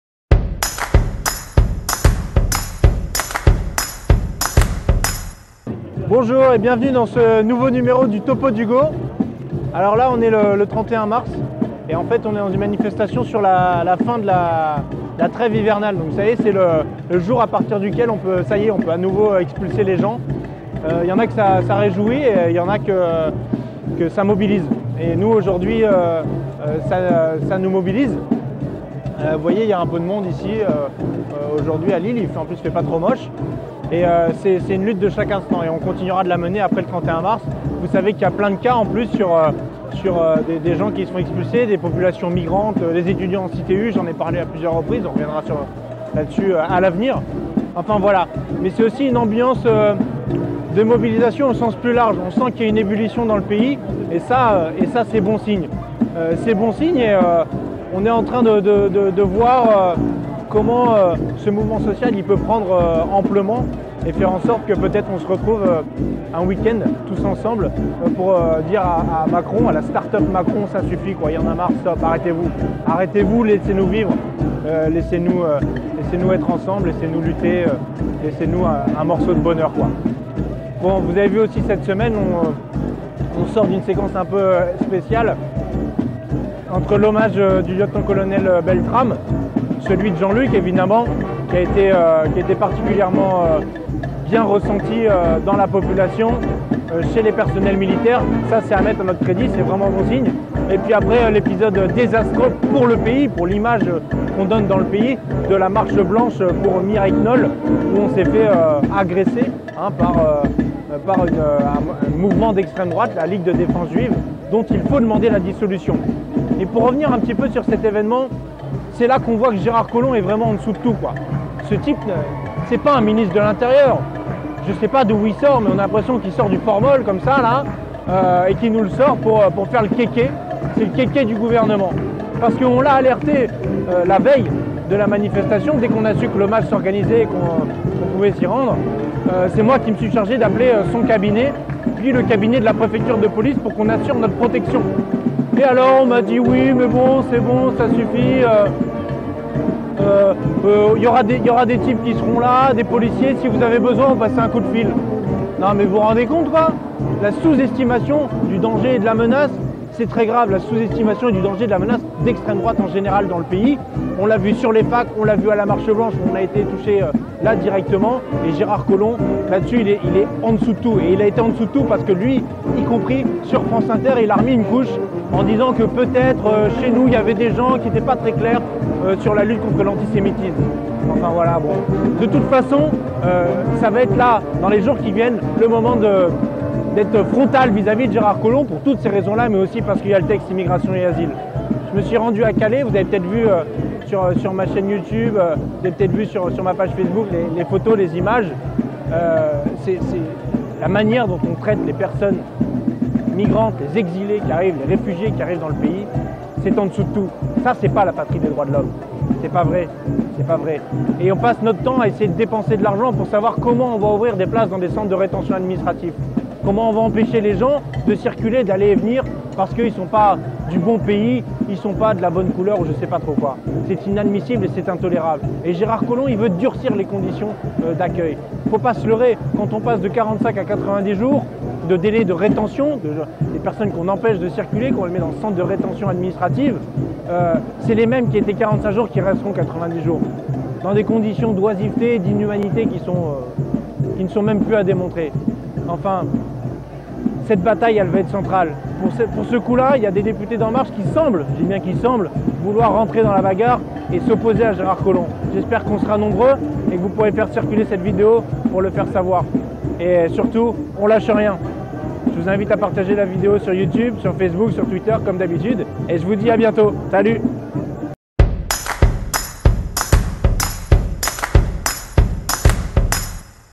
Le Topo d'Ugo #37 - "Mobilisés pour rassembler toutes les luttes"
Mobilisé contre les expulsions locatives, je prends le temps de vous parler de Gérard Collomb.